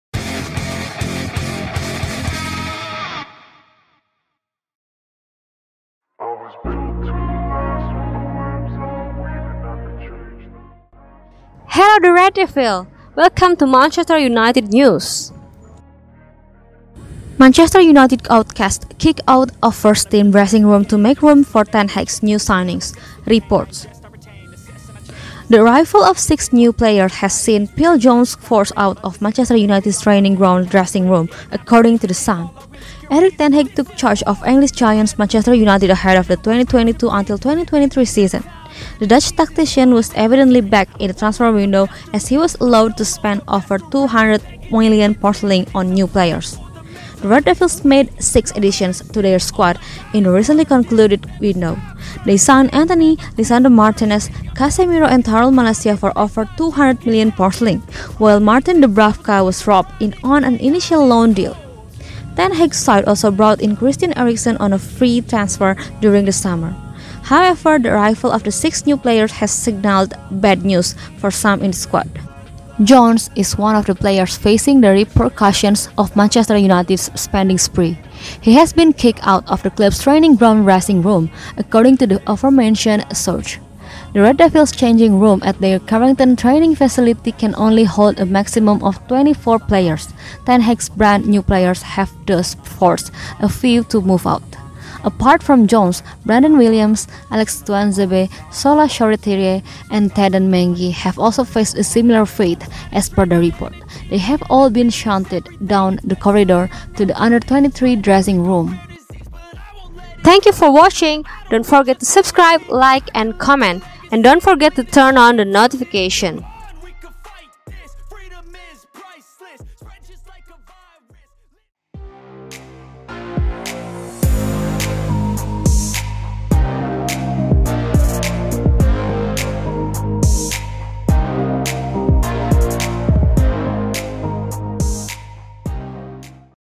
SHOCKING NEWS❗ Man United outcasts sent off first team dressing room to make room for new players
The arrival of six new players has seen Phil Jones forced out of Manchester United's training ground dressing room, according to The Sun.
Erik ten Hag took charge of English giants Manchester United ahead of the 2022-23 season. The Dutch tactician was evidently backed in the transfer window as he was allowed to spend over £200 million on new players.